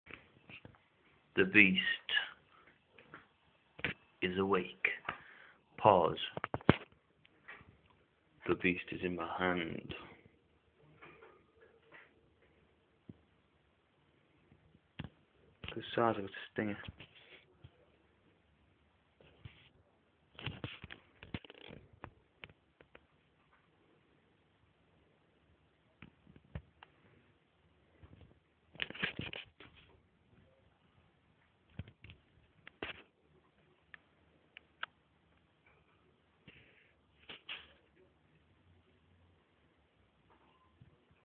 me holding my scorpion
just a little video of me holding my scorpion.